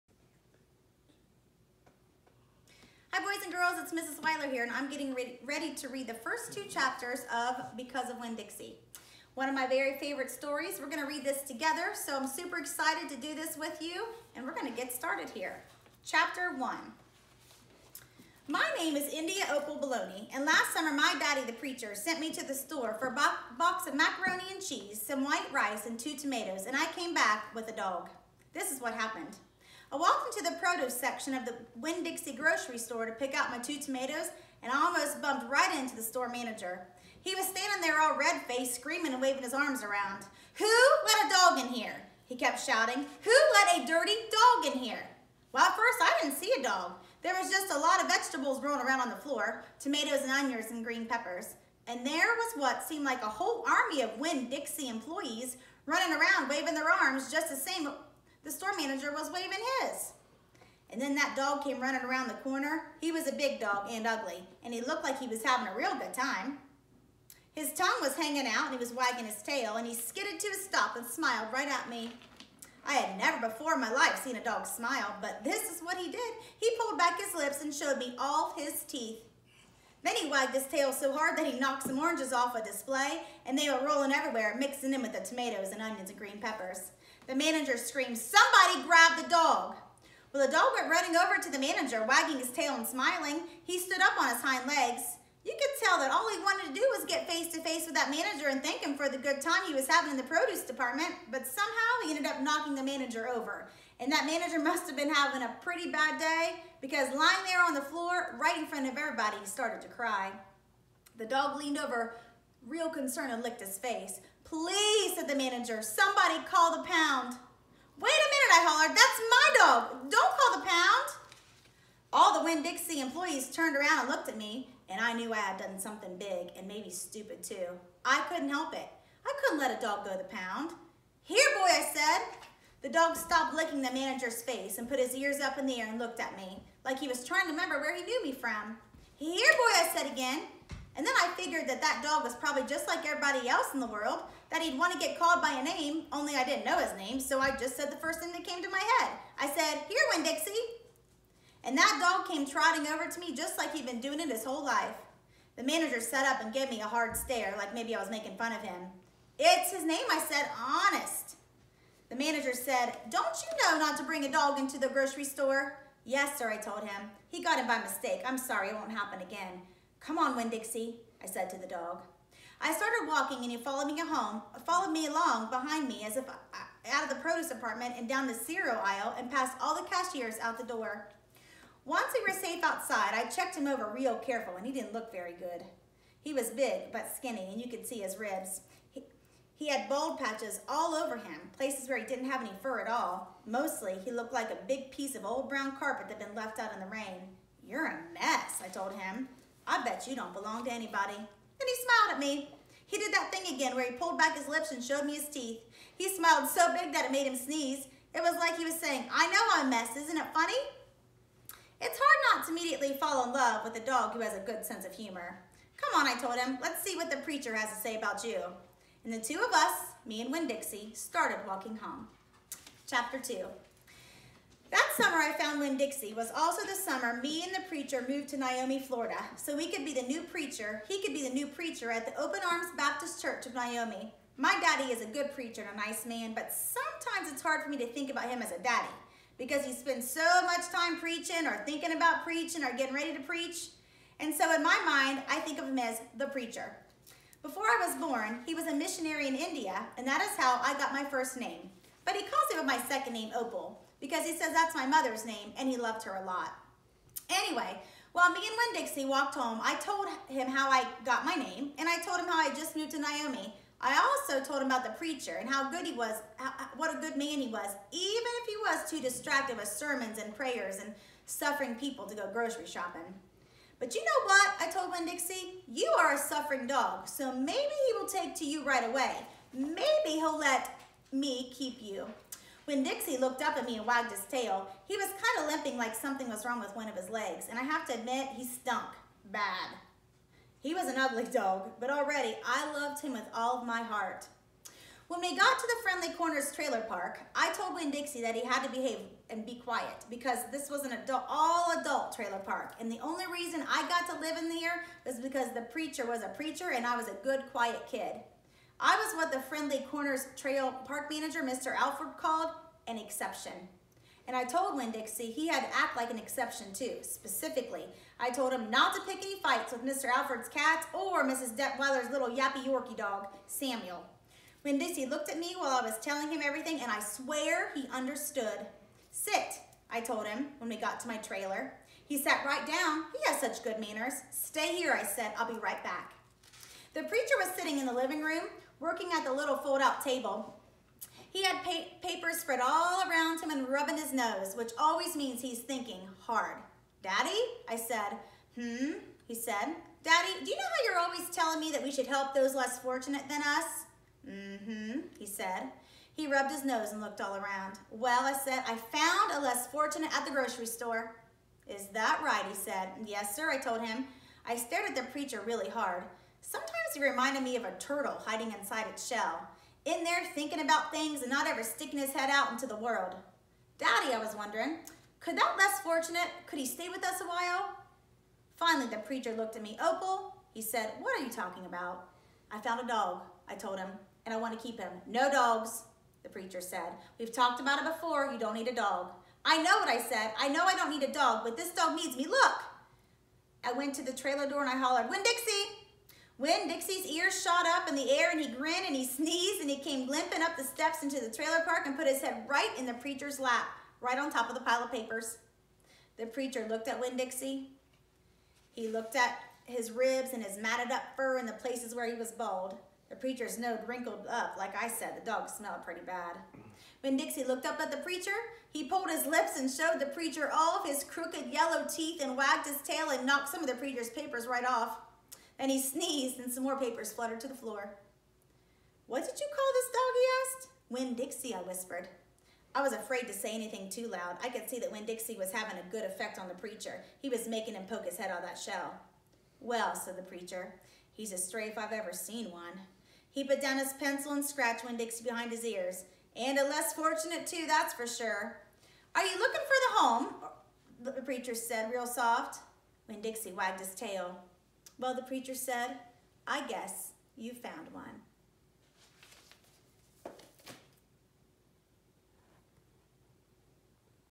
Because of Winn-Dixie Ch 1 and 2
Read by Mrs. Weiler, Principal